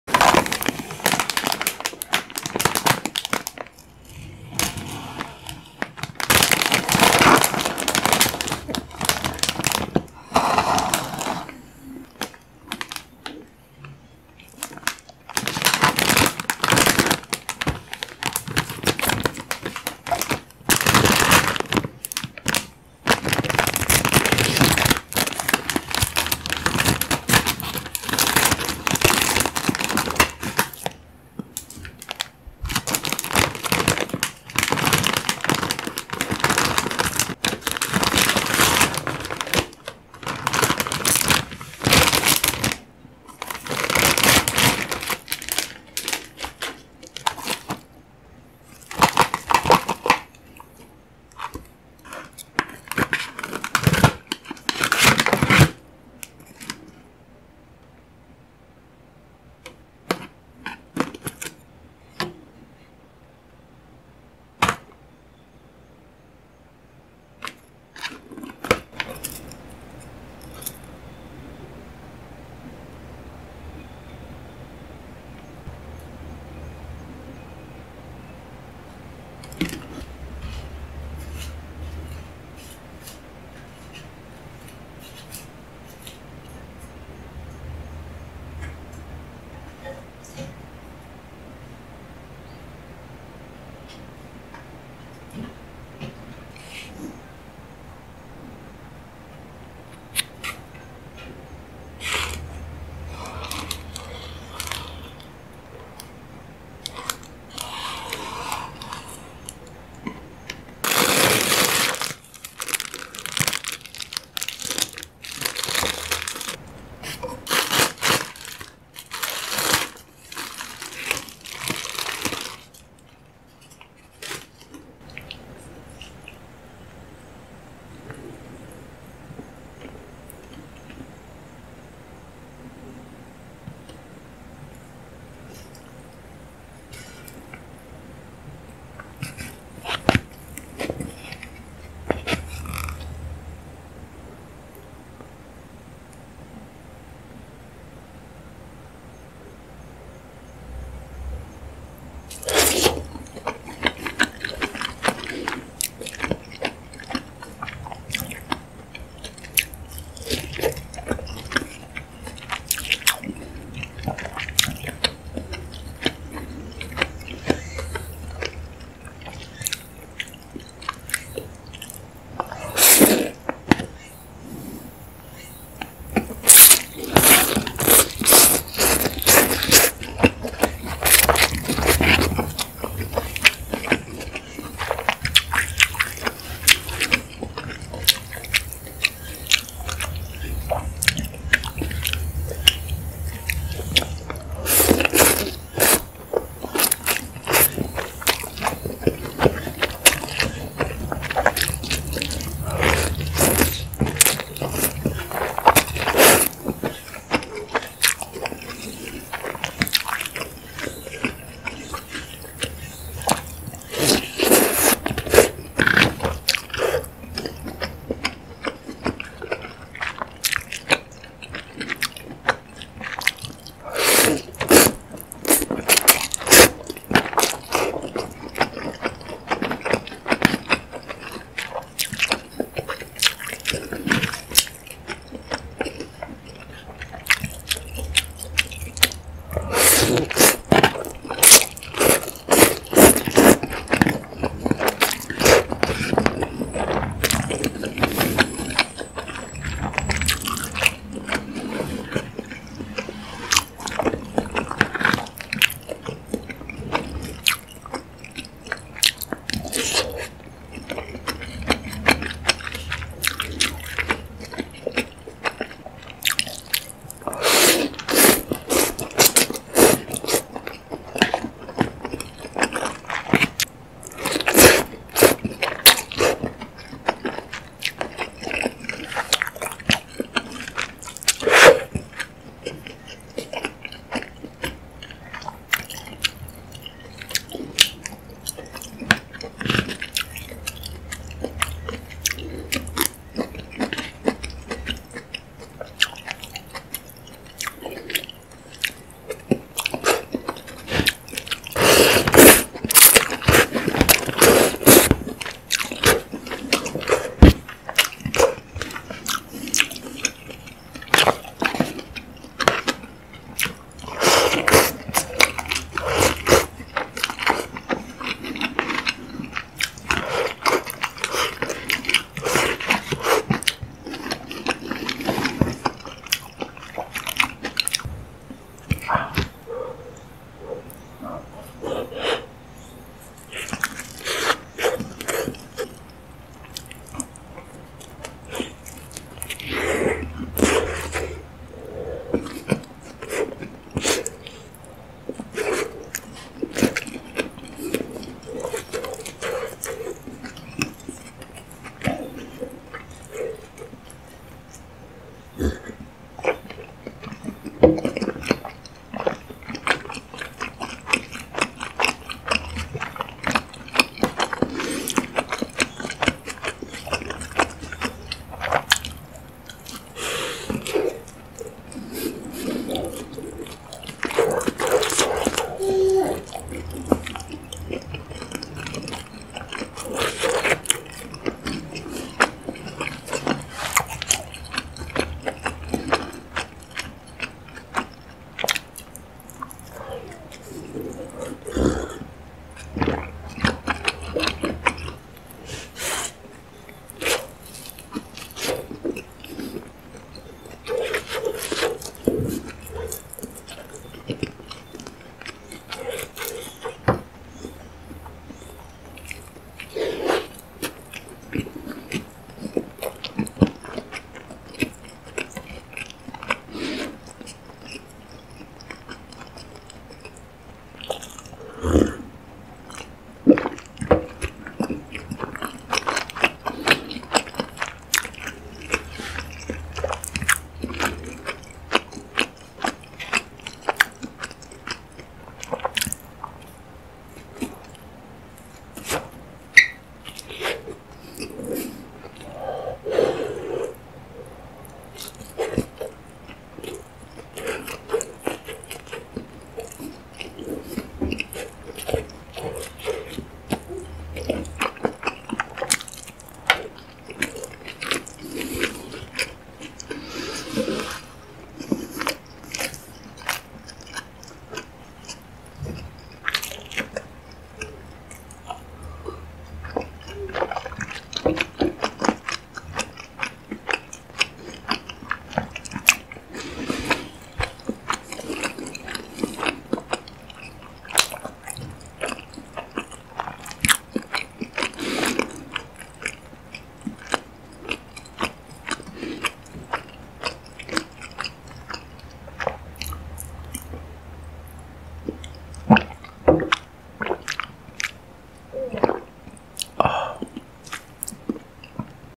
ASMR MUKBANG Japanese canned noodles | Won TV ASMR #8
ASMR MUKBANG  review Japanese canned noodles | Won TV ASMR #8

Won TV ASMR